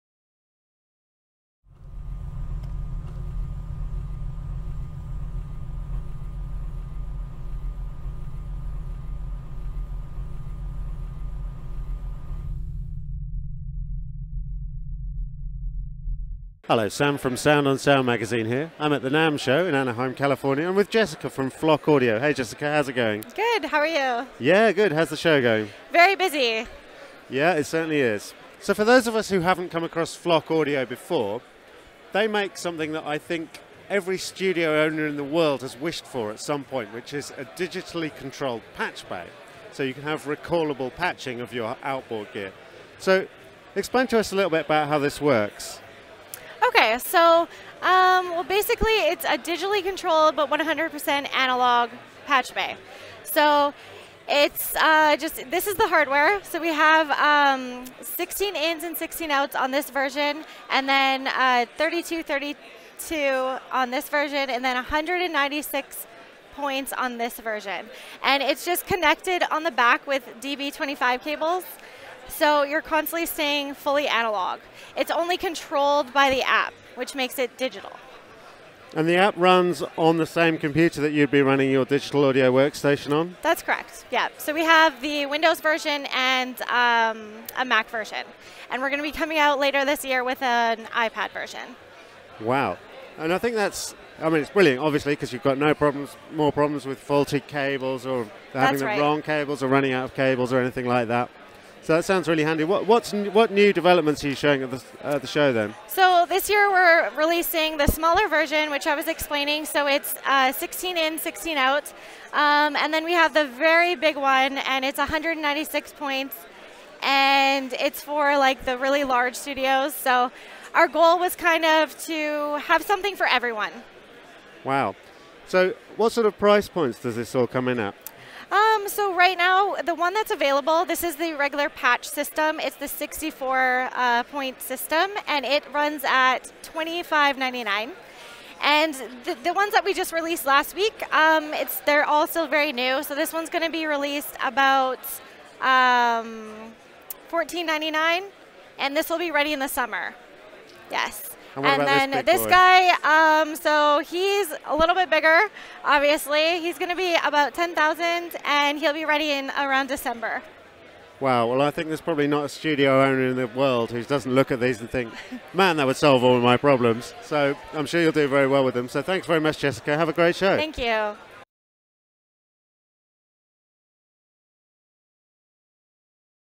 Flock Audio Patch LT and XT - NAMM 2020
Flockj Audio announce smaller and larger Patch digital patch bays, the LT and XT.

Follow Sound On Sound on Instagram: @soundonsoundmag